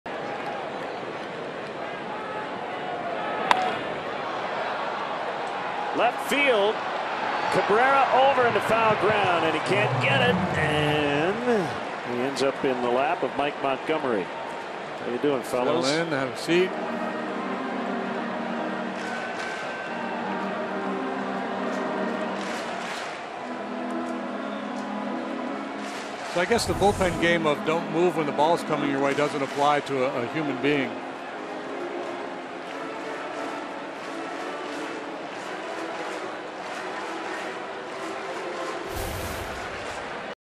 CWS@CHC: Melky takes a seat with the Cubs' bullpen
Melky Cabrera runs down a fly ball in foul territory but it bounces into the crowd, so he decides to take a seat with the Cubs' bullpen